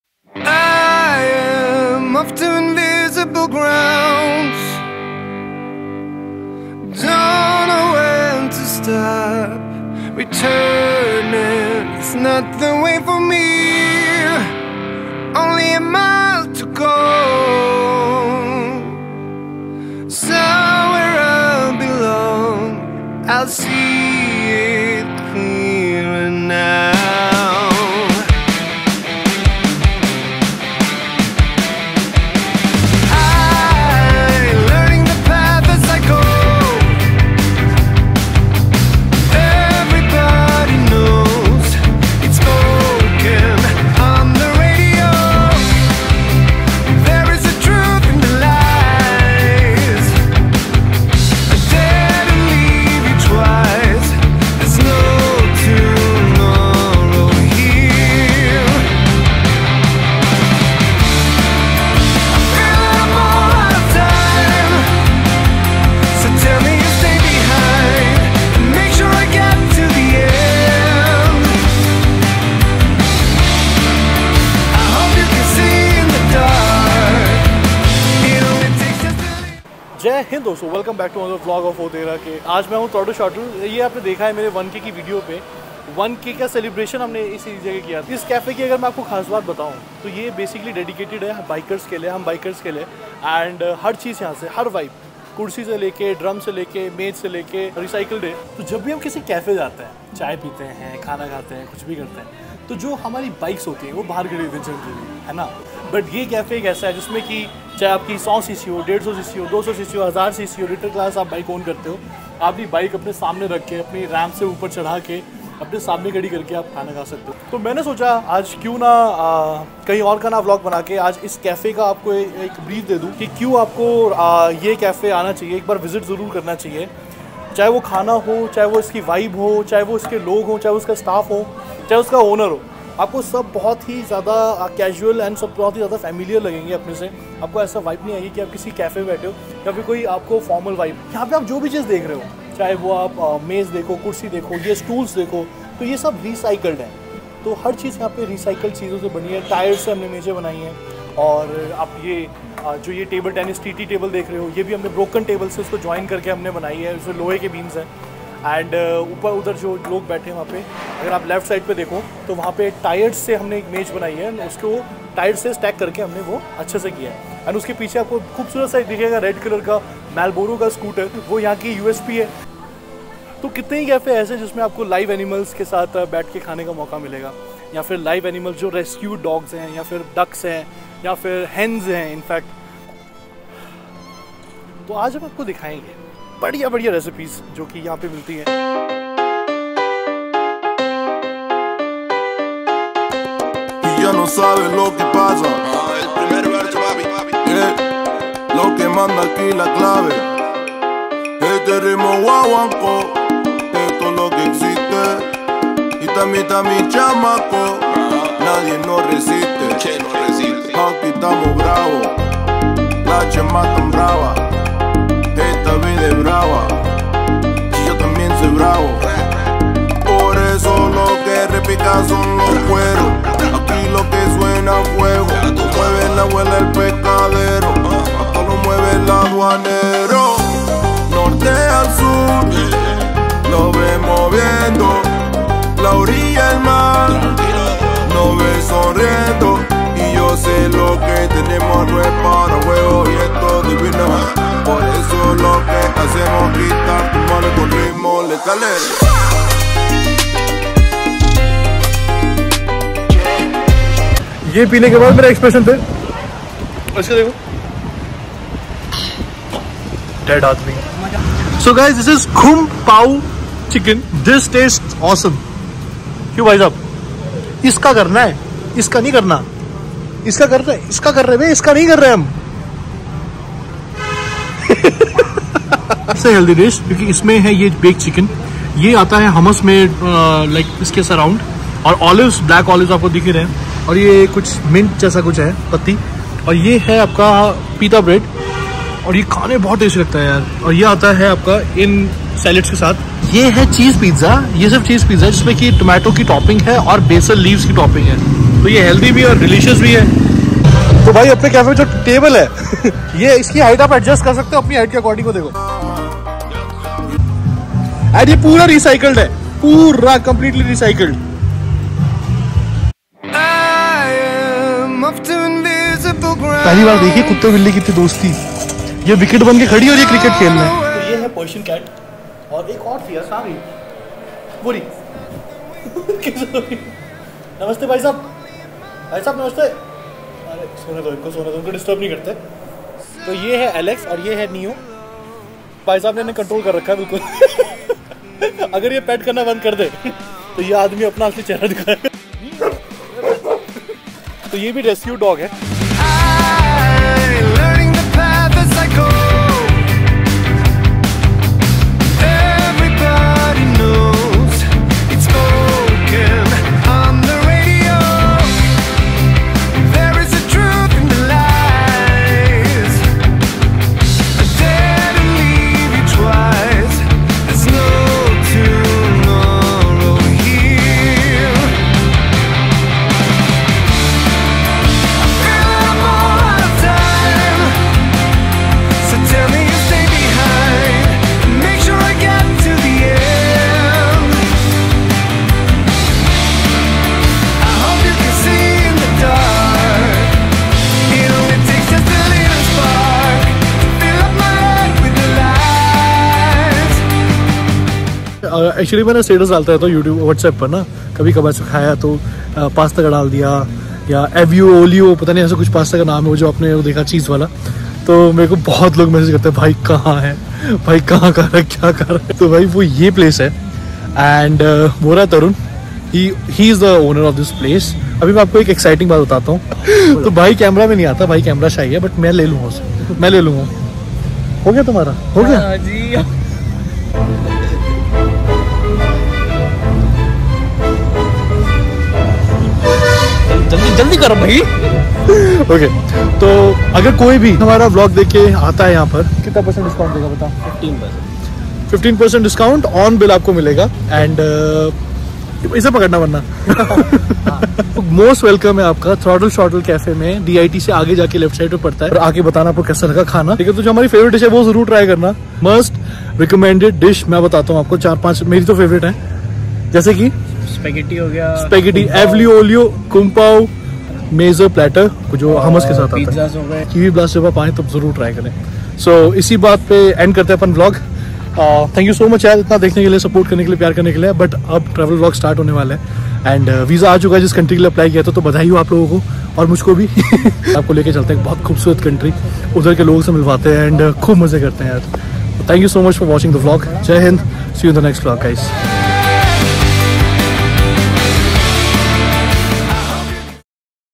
Amazing cafe in Dehradun || Throttle Shrottle || Oooterakee
Motocafe which is made of recycled material and serve a variety of cuisine one of its kind in Dehradun.